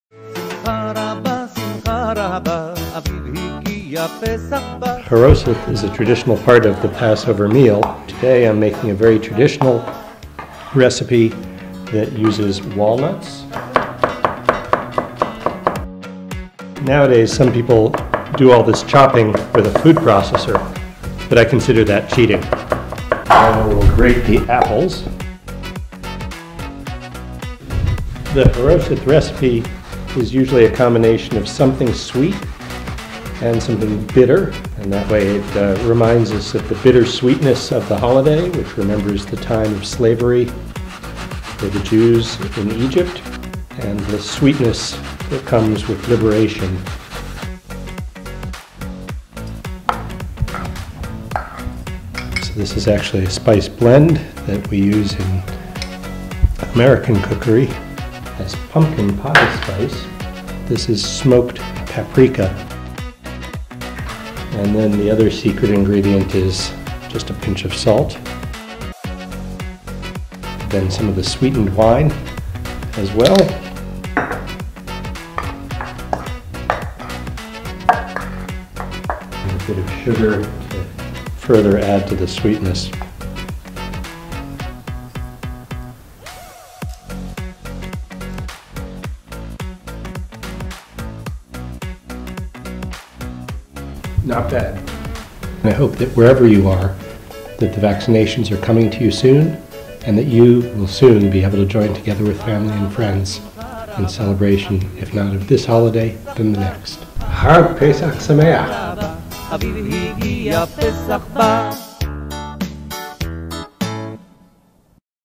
Happy Passover from the U.S. Chargé d'Affaires in Israel
מהו המרכיב הסודי במתכון החרוסת של הממונה על ענייני שגרירות ארה״ב בישראל, ג׳ונתן שרייר? התשובה בסרטון...
האם גם לכם יש מתכון מנצח לחרוסת? מוזמנים לשתף אותו איתנו בתגובות!
חג פסח שמח!🌸

What’s our Chargé d'Affaires secret Charoset ingredient? Watch and find out. Do you have your own secret recipe? Share it with us in the comments below!
Happy Passover! 🌸